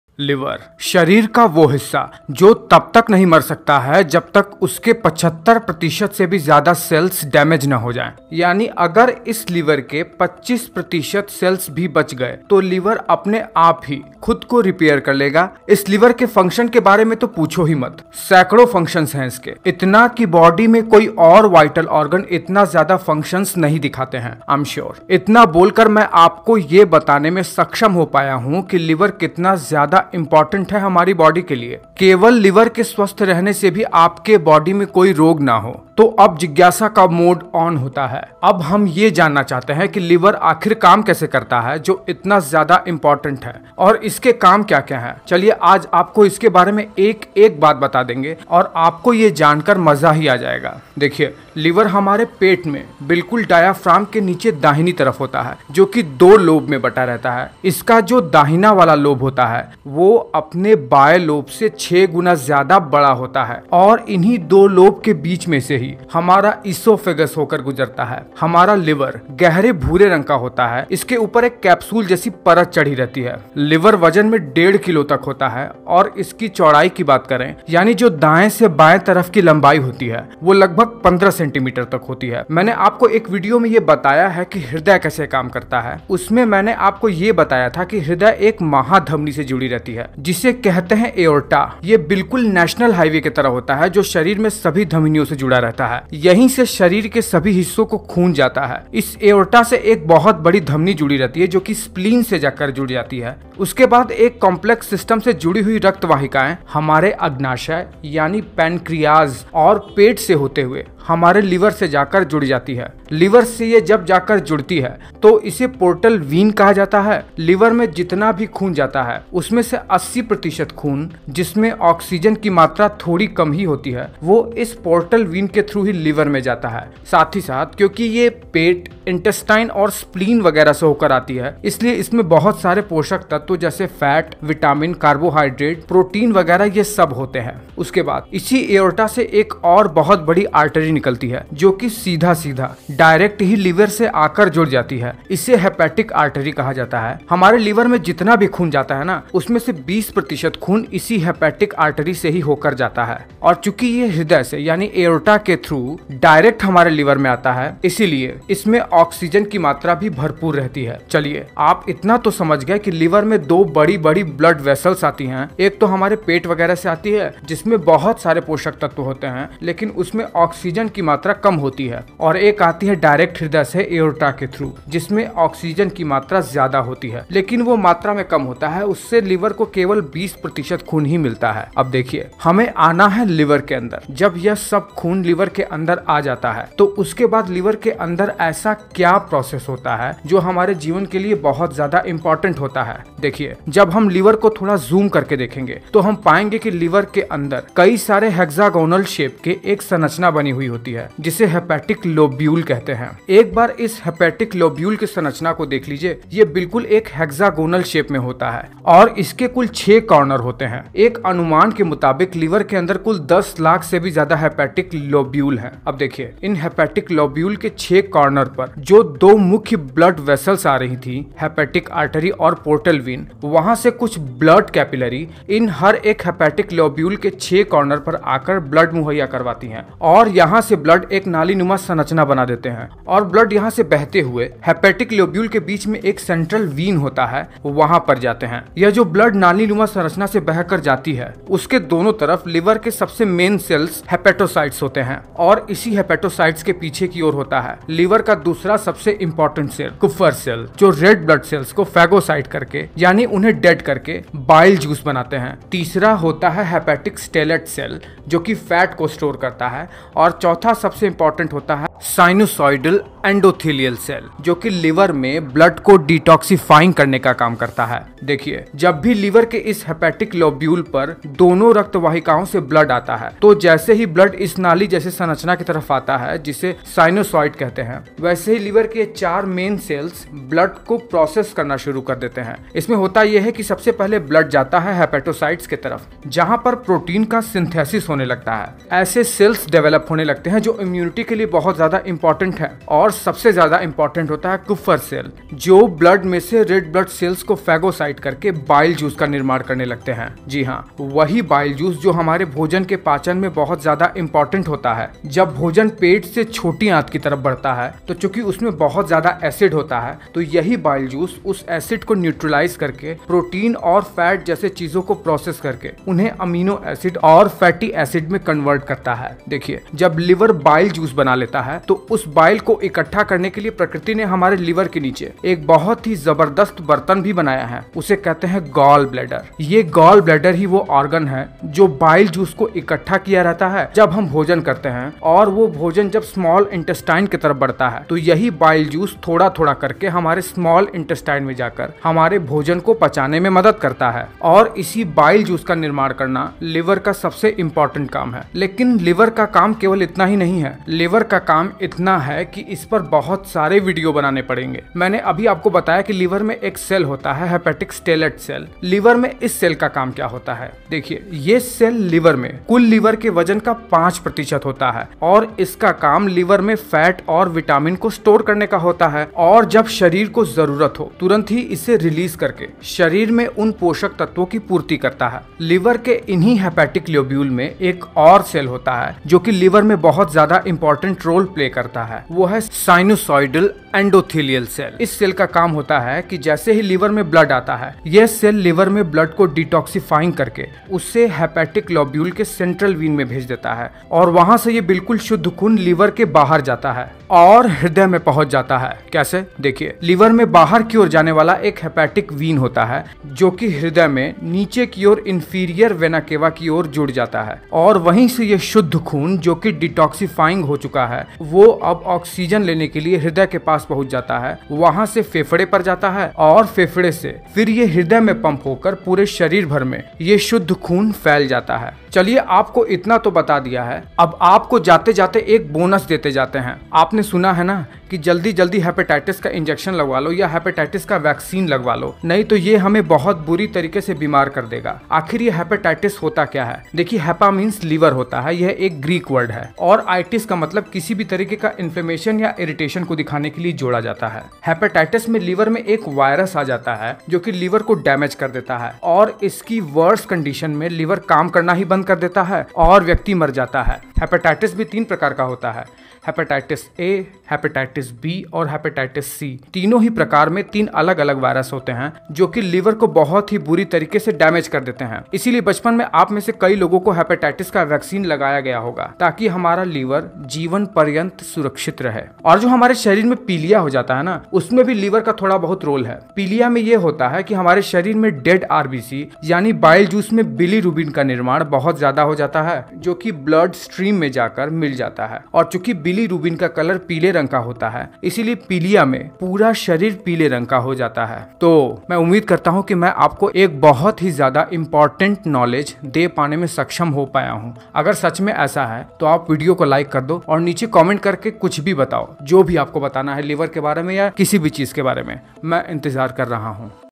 लिवर कैसे काम करता हैं - digestive system 8 - how does liver work in hindi
video copyright - Vidyudabhi